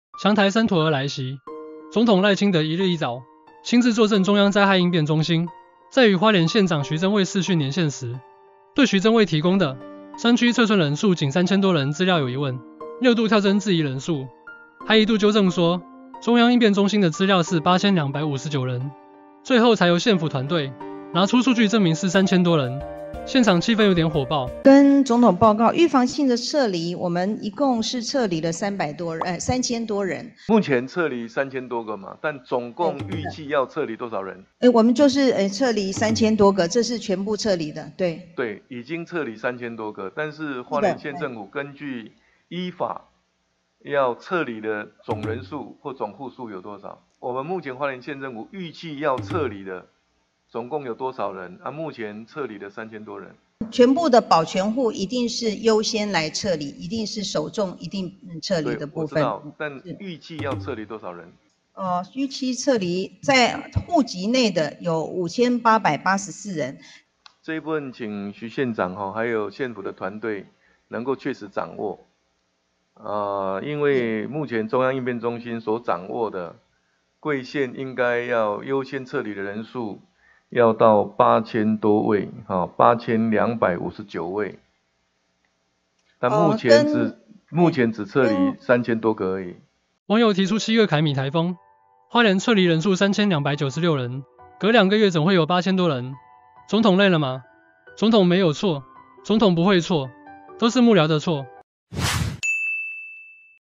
總統賴清德狂跳針！6問徐榛蔚撤離人數 網酸: 總統不會錯，錯的都是幕僚｜品觀點
統賴清德1日到中央災害應變中心聽取防颱整備狀況，與花蓮縣長徐榛蔚視訊時，因撤村人數與縣府資料不同，六度跳針質問徐榛蔚，質疑花蓮縣未確實掌握狀況，但事後證明花蓮縣提供的數字是對的.
網友: 總統不要生七七，總統永遠都是對的！因為錯的都是幕僚！！

時事議題 多元角度觀點  全都在品觀點！